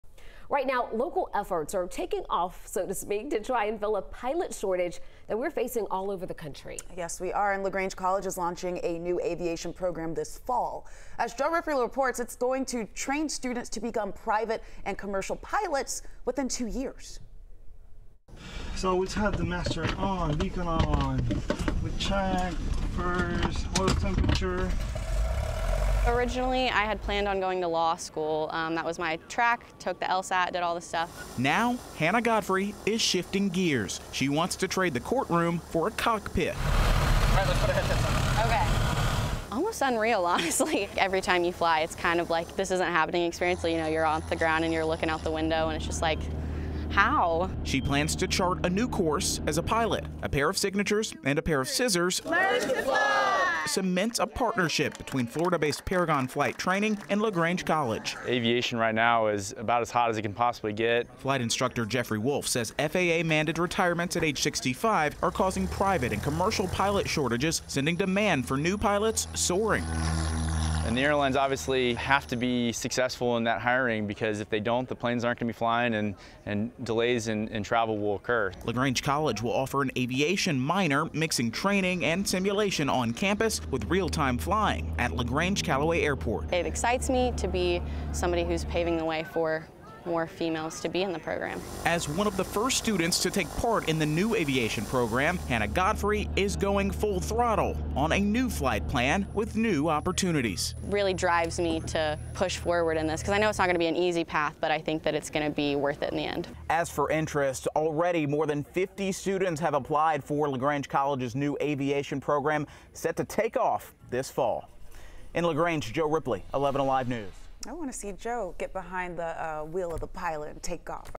New aviation program takes off at LaGrange College to help with pilot shortage
The required retirement age at 65 is causing private and commercial pilot shortages, causing the demand to soar.